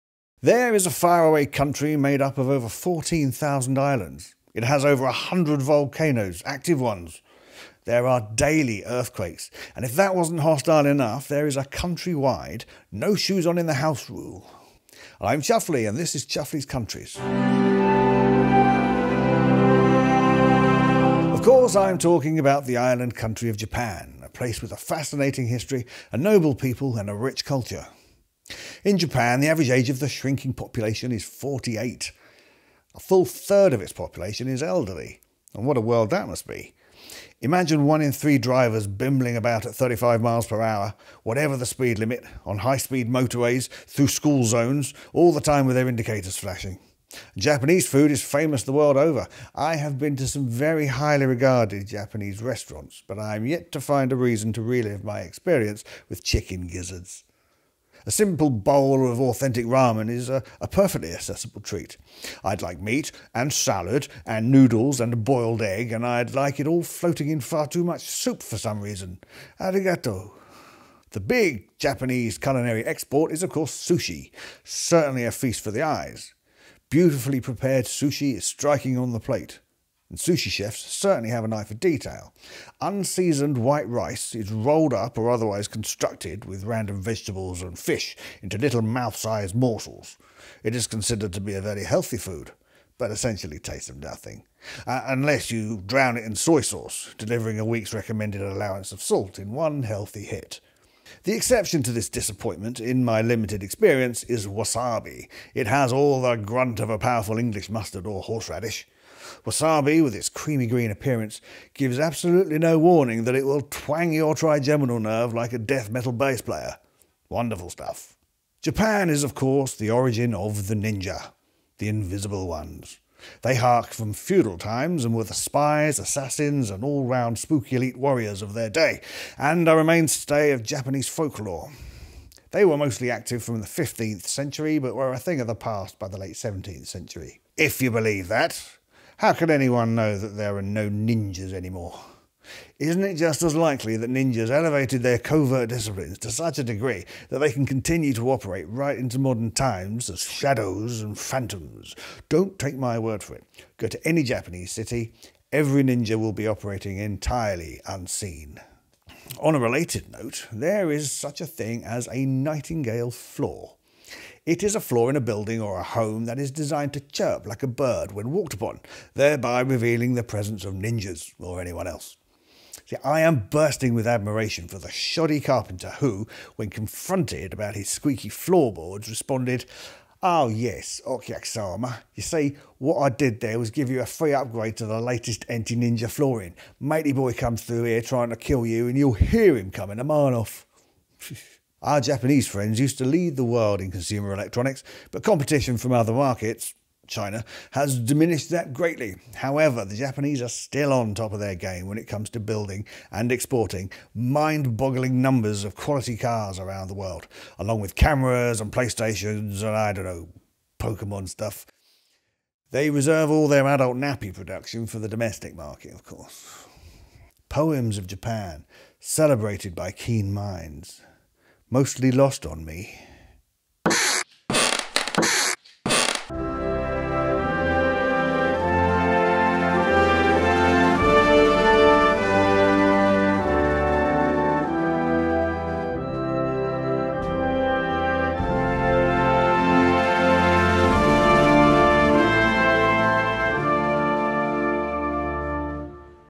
Japan! Chuffley's Countries examines the Land of the Rising Sun
Toyotas and raw fish! Old people and floorboards!

There is a faraway country made up of over 14000 islands. It has over a 100 active volcanoes. There are daily earthquakes. If that wasn’t hostile enough, there is a country-wide “no shoes on in the house” rule.
Of course I’m talking about the Island Country of Japan, a place with a fascinating history, a noble people and a rich culture.
In Japan, the average age of the shrinking population is 48. A full third of its population is elderly. What a world that must be. Imagine one in three drivers bimbling about at 35 miles per hour whatever the speed limit. On high-speed motorways, through school zones, always with their indicators flashing.
Japanese food is famous the world over. I have been to some very highly regarded Japanese restaurants but I’m yet to find a reason to relive my experience with chicken gizzards. A simple bowl of authentic ramen is a perfectly accessible treat - I’d like meat and salad and noodles and a boiled egg, please and I’d like it all floating about in far too much soup for some reason. Arigato.
The big Japanese culinary export is of course sushi. Certainly a feast for the eyes, beautifully prepared sushi is striking on the plate. Sushi chefs certainly have an eye for detail. Unseasoned white rice is rolled up or otherwise constructed with random vegetables and fish into little mouth-sized morsels. It is considered to be a very healthy food, but essentially tastes of nothing unless you drown it in soy sauce, delivering a week’s recommended allowance of salt in one healthy hit. The exception to this disappointment in my limited experience is wasabi. It has all the grunt of a powerful English mustard or bold horseradish. Wasabi, with its creamy-green appearance, gives no warning that it will twang your trigeminal nerve like a death metal bass player. Wonderful stuff.
Japan is of course known as the origin of the ninja. The invisible ones. They hark from feudal times and were the spies, assassins, and all-round spooky elite warriors of their day, and are a mainstay of Japanese folklore. They were mostly active from the 15th century but were a thing of the past by the late 17th century. IF YOU BELEIVE THAT! How can anybody know that there are no more ninjas? Isn’t it just as likely that ninjas elevated their covert disciplines to such a degree that they can continue to operate right into modern times as shadows and phantoms. Don’t take my word for it! Go to any Japanese city. Every ninja will be operating entirely unseen.
On a related note, there is such a thing as a nightingale floor. It’s a floor in a building or home that is designed to chirp like the bird when walked upon, thereby revealing the presence of ninjas (or anyone else). I am bursting with admiration for the shoddy carpenter who, when confronted about his squeaky floorboards responded with “Oh yes, Okyakusama “ok yak sama”, you see what I did there was give you a free upgrade to the latest anti-ninja flooring. Matey-boy comes through here trying to kill you and you’ll hear him coming a mile off.”
Our Japanese friends used to lead the world in consumer electronics, but competition from other markets - cough china - has diminished that greatly. However they are still on top of their game when it comes to building and exporting mind-boggling numbers of quality cars around the world along with cameras, and PlayStations, and I don’t know: pokemon shit? They reserve all their adult nappy production for their domestic market, of course.

Poems of Japan
Celebrated by keen minds
Mostly lost on me